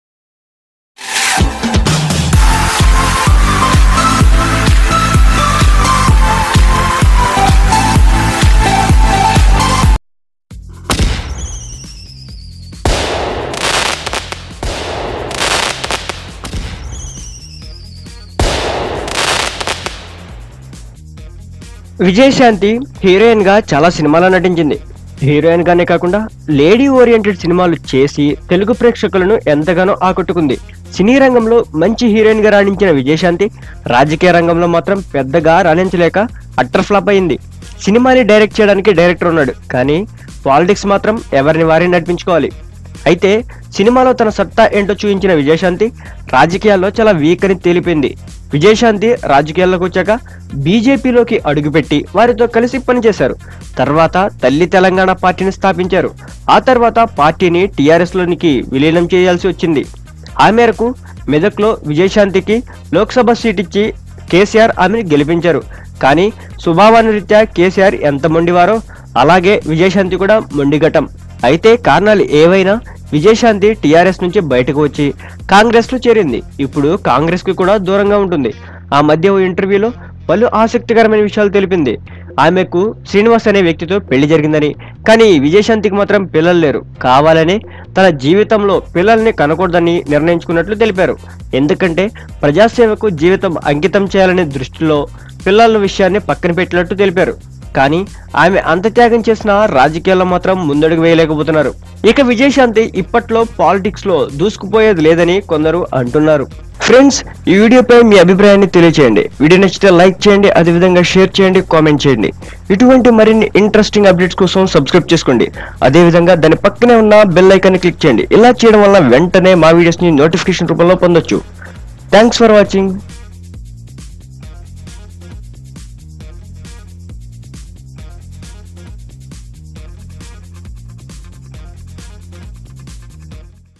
Reason Why Vijayashanti Does NOT have KIDS | విజయశాంతి పిల్లల్ని కనకపోవడానికి కారణం ఏంటో తెలుసా??
Reason Why Vijayashanti Does NOT have KIDS revealed in this video on Trend Setter.